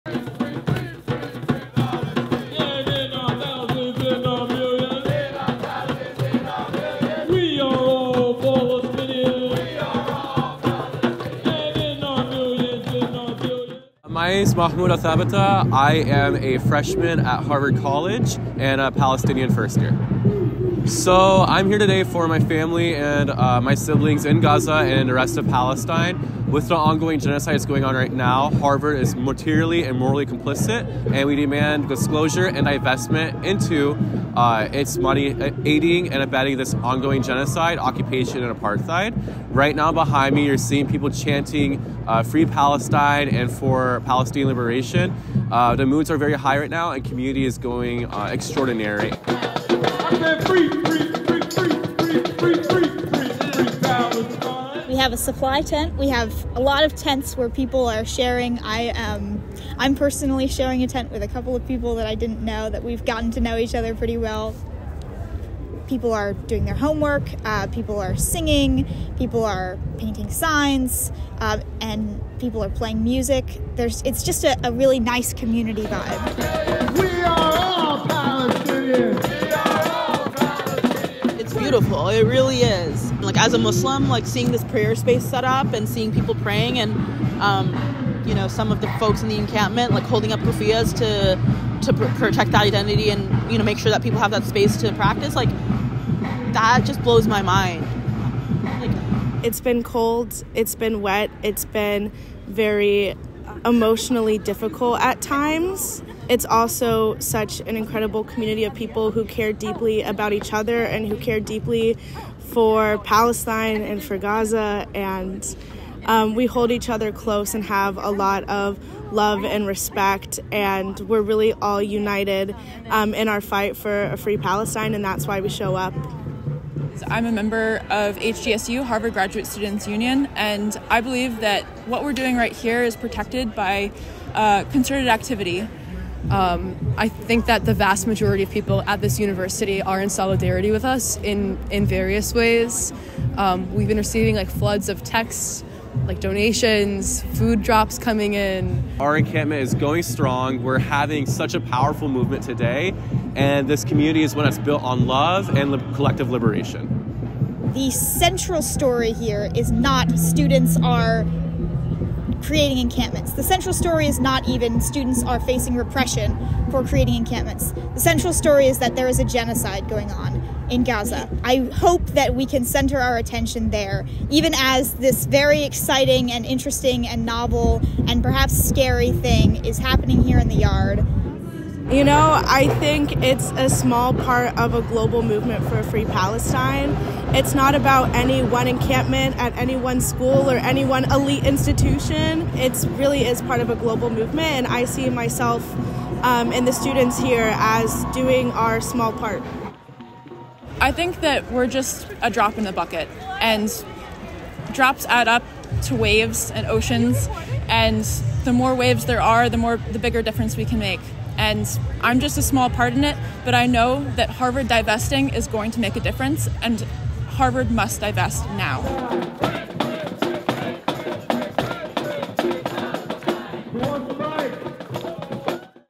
Student Perspectives From the Pro-Palestine Harvard Yard Encampment
Three days after pro-Palestine organizers began
 an encampment in Harvard Yard, The Crimson
asked students why they’re participating and
what their experience has been. Here is what they said.

Video by Ellen P. Cassidy and Julian J. Giordano